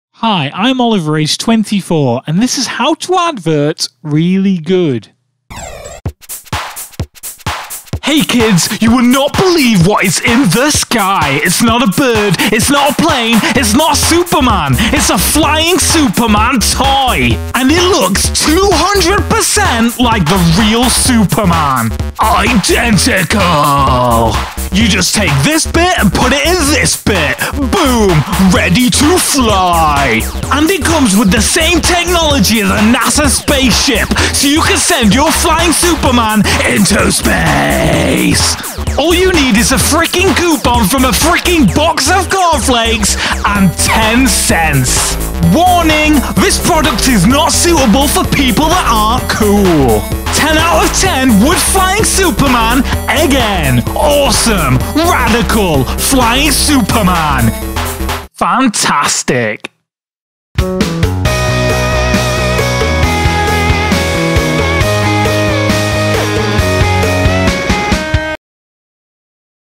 How To Advert Really Good - Flying Superman
My name is Oliver and I'm 24. Here is a video I filmed to help my friends, fans and family advert. I hope you enjoy it and make many beautiful adverts's that are professional with 4D effect like mine.